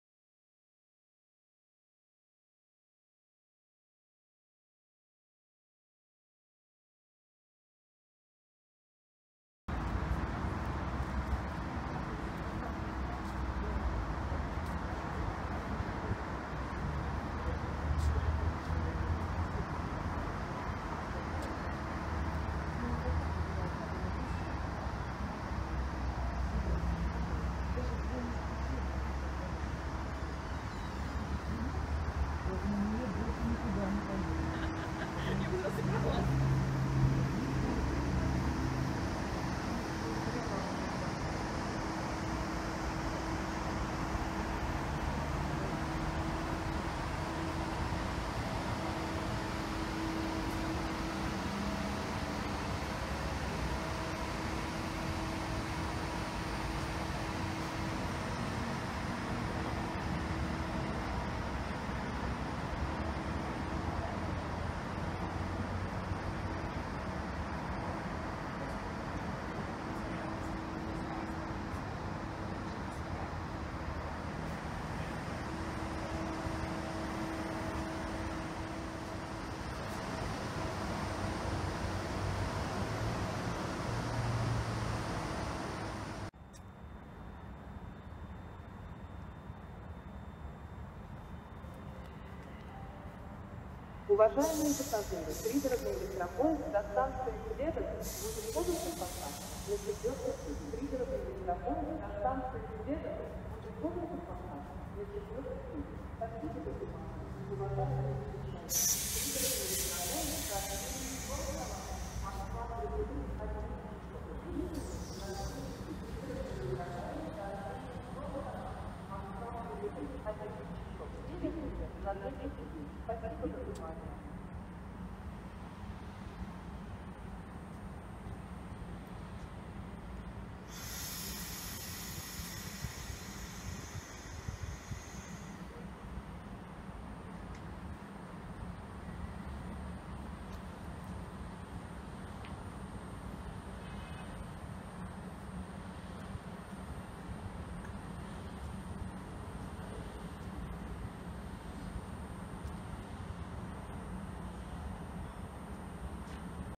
Электропоезд ЭГ2Тв-038 на Рижском вокзале.
Электропоезд ЭГ2Тв-038 построен в 2019 году на Тверском вагоностроительном заводе, приписан к Частным перевозчикам, депо АО «Центральная пригородная пассажирская компания».